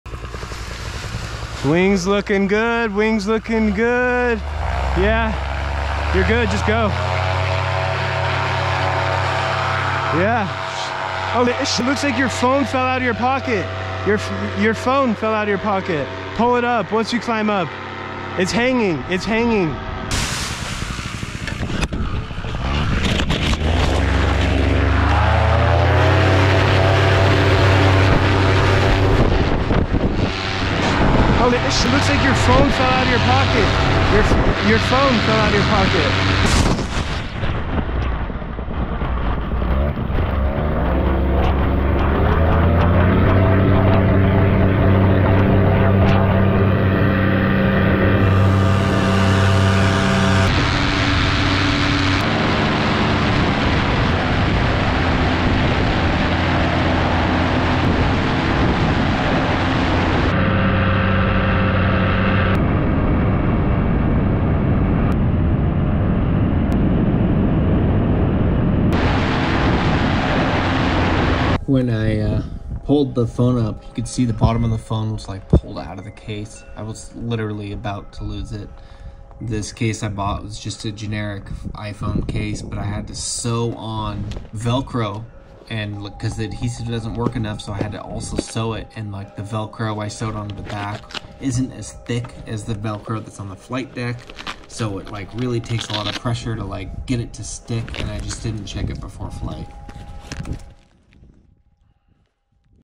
Phone fell out of pocket during Paramotor Launch - Abort - Crash?
Doing a Powered Paraglider forward launch, I didn't secure my phone to my Gin flight deck before taking off. My flight buddy Bryce Mann @brycemannn4847 told me my phone was dangling. I had to climb and get more altitude before letting go of my brakes and throttle before pulling my phone up. Luckily, it didn't fall out even though it was practically already out of its case by the time I grabbed it. Luckily this didn't lead to a Paramotor Crash or Paragliding Launch Abort flight disaster.